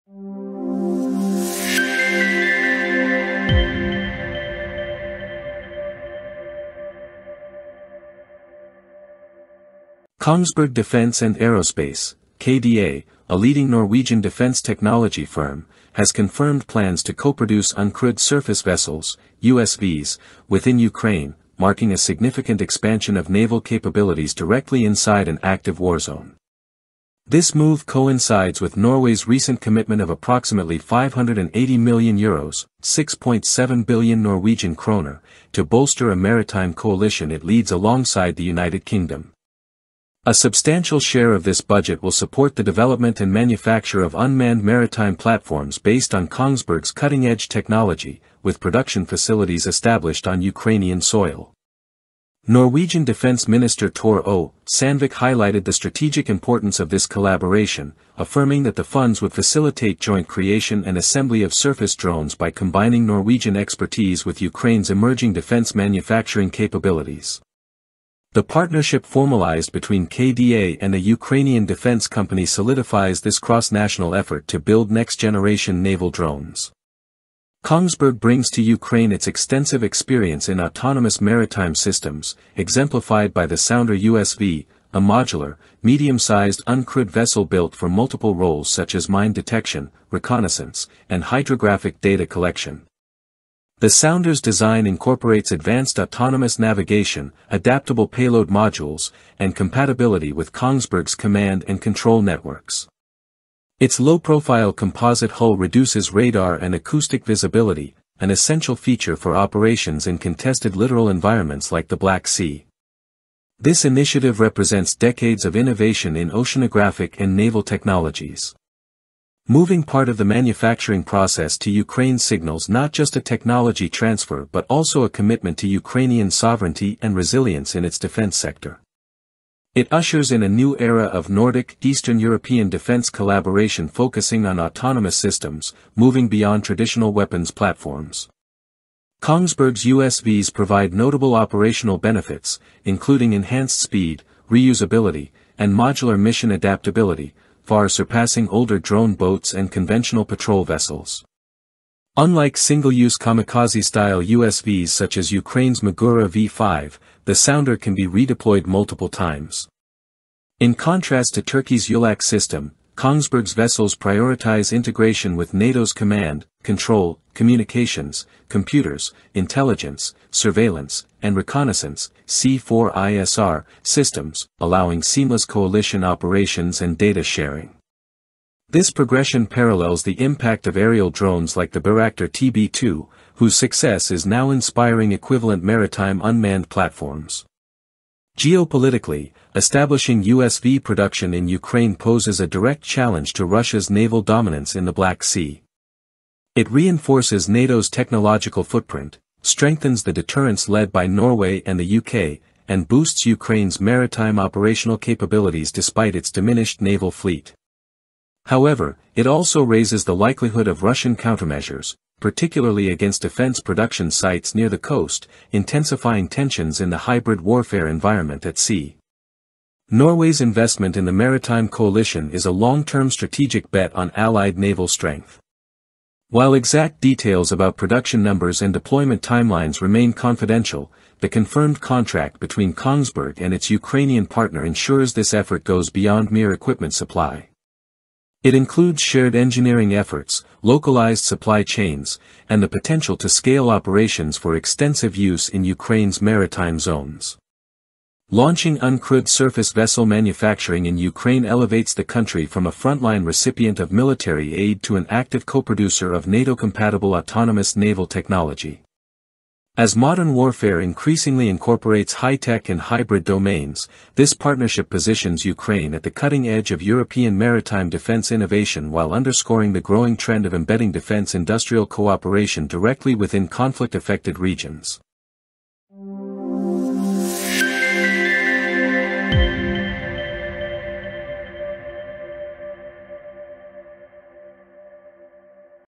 Norway and Ukraine Team Up to Build Advanced Uncrewed Naval Vessels Amid Conflict
Norway’s Kongsberg and Ukraine launch co-production of uncrewed surface vessels (USVs) amid war, enhancing NATO-compatible naval tech. The partnership boosts Ukraine’s defense industry and challenges Russian dominance in the Black Sea.